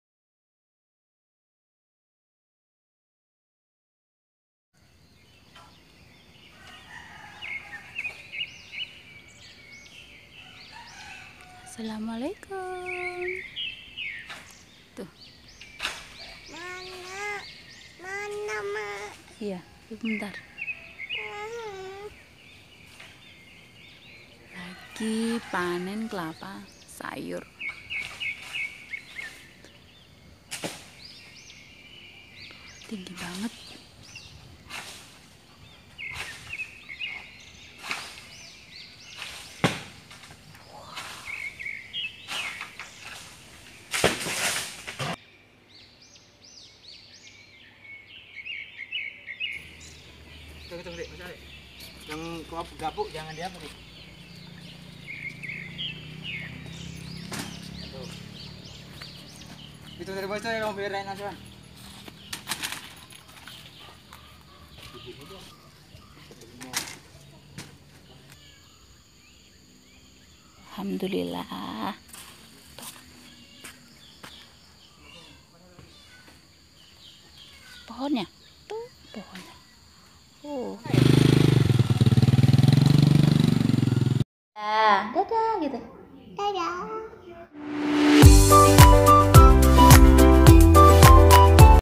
PANEN KELAPA TUA.. kira2 dapat berapa ya??..
Assalamualaikum..
Alhamdhulillah ya pagi2 ada yg beli klapa sayur tua dapat 125 buah..
Kira2 dibeli berapa ya..??
Karna terlampau tinggi pohony g bisa di panen..
Kebetulan yg beli manen sendiri kupas sendiri kita kasi harga Rp2.000 Harga klapa parut disini Rp 6.000
125 buah × 2.000=Rp 250.000 alhamdhulillah..
Jadilah pribadi yg pandai bersyukur itu akan membuat hidupmu tentram..

Rekam vidio: Hp samsung J7 Prime
EDIT               :KINEMASTER